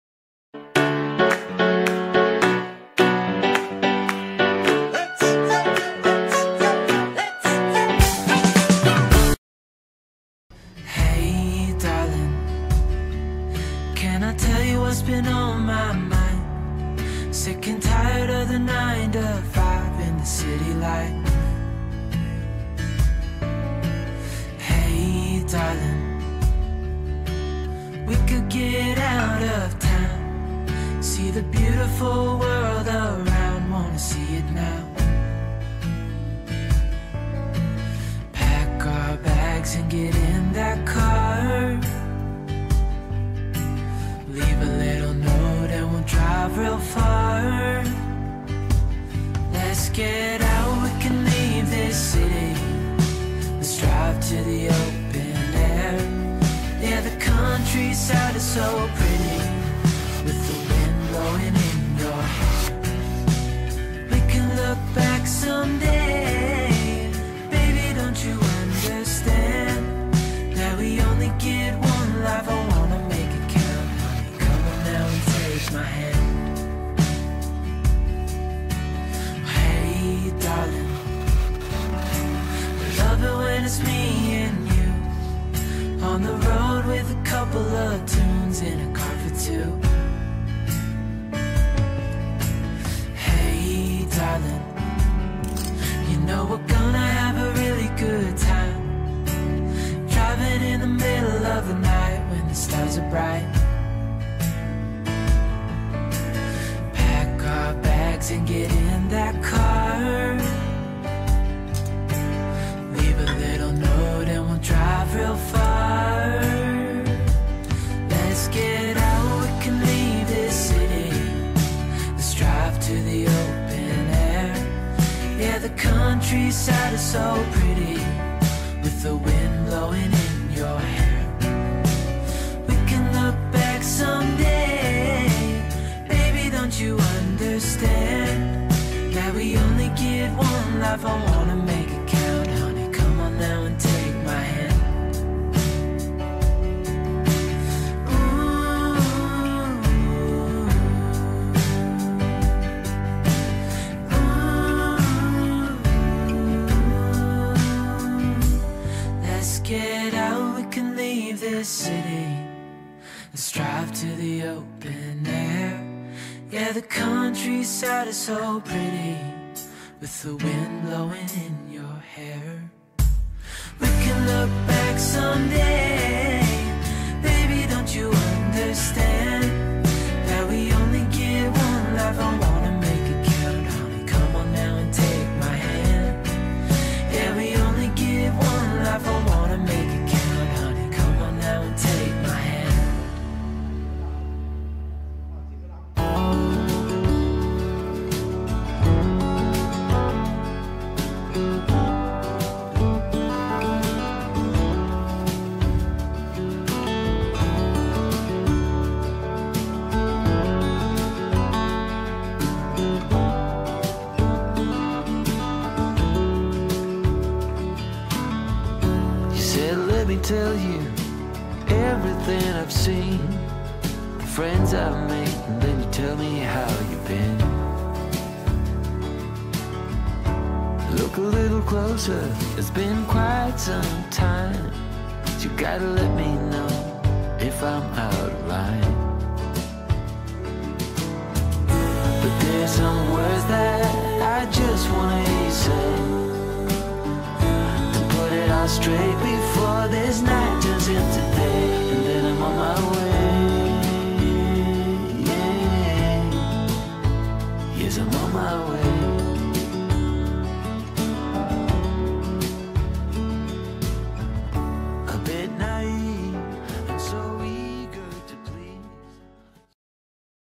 Absolutely Gorgeous Black Wedge Park Model from Champion Homes
Size: 399 Square Feet, 1 Bedrooms, 1 Bathrooms, Park Model RV

This Park Model RV is built to the ANSI code.

Music Background: